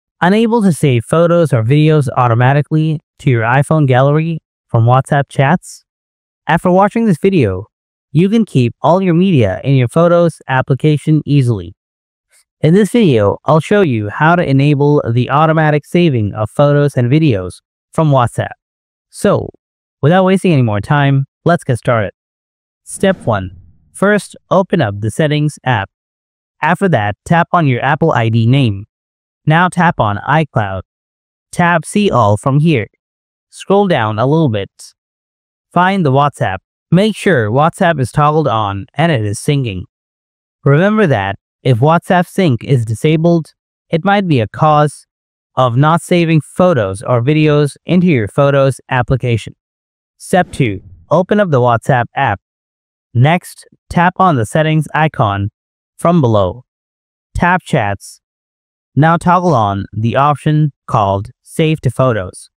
How to Save WhatsApp Photos or Videos Automatically on iPhone | Auto Download Media on iOS
Want WhatsApp to save all your media directly to your iPhone? In this video, I’ll show you how to automatically save WhatsApp photos and videos on iPhone. Whether you want to auto-download images or store videos in your gallery, this step-by-step guide will help you enable the right settings.

You’ll learn how to turn on WhatsApp auto-download for media, save media to your camera roll, and manage storage to avoid clutter. Perfect for anyone who doesn’t want to manually save each file.

Watch now to make WhatsApp photos and videos save automatically on your iPhone!